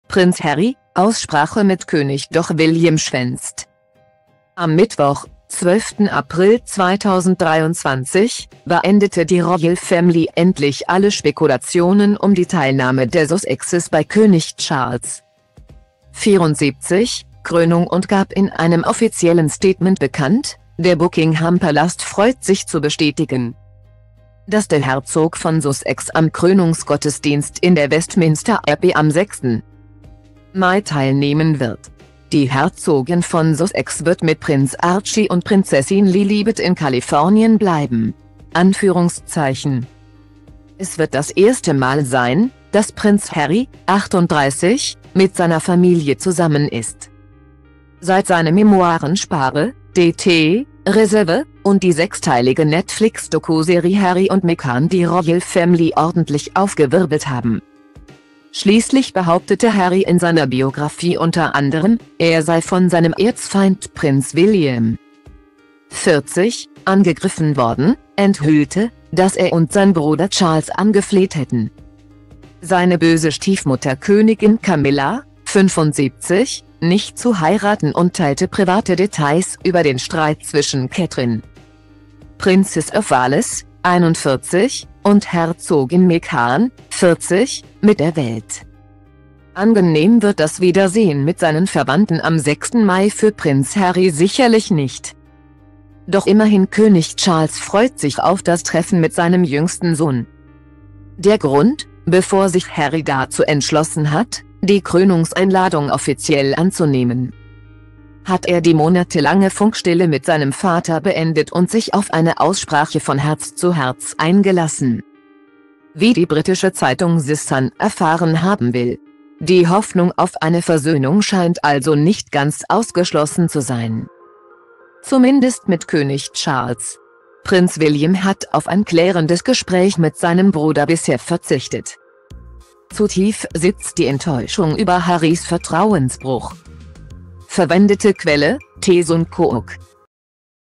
Prinz Harry: Aussprache mit König – doch William schwänzt
Am Mittwoch, 12. April 2023, beendete die Royal Family endlich alle Spekulationen um die Teilnahme der Sussexes bei König Charles',74, Krönung und gab in einem offiziellen Statement bekannt: "Der Buckingham Palast freut sich zu bestätigen,dass der Herzog von Sussex am Krönungsgottesdienst in der Westminster Abbey am 6.